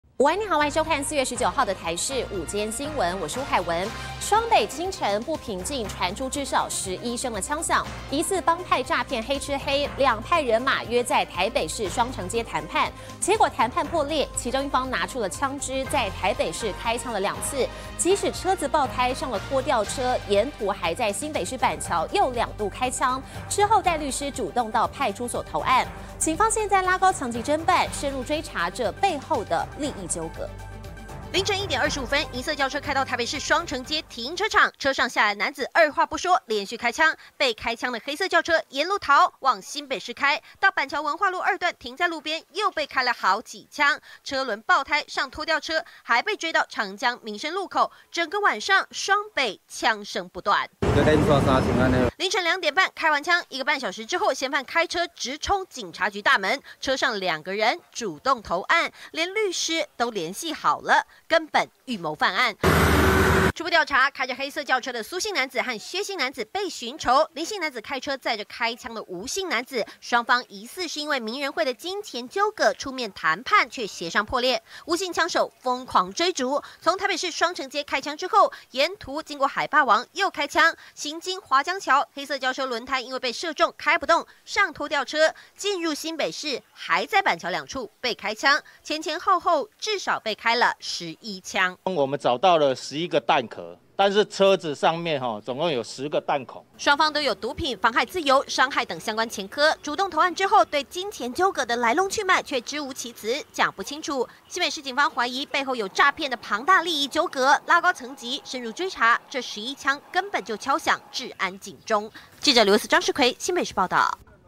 疑幫派詐騙黑吃黑 談判破裂開至少11槍
雙北今天(19)清晨不平靜，傳出至少11聲槍響！疑似幫派詐騙黑吃黑，兩派人馬約在台北市雙城街談判，沒想到，談判破裂，其中一方拿出槍枝拼命追，在台北市開槍兩次，即使車子爆胎上了拖吊車，還沿途在新北市板橋又兩度開槍，一個半小時後，帶律師主動去派出所投案，根本是預謀犯案。警方拉高層級偵辦，深入追查背後的利益糾葛。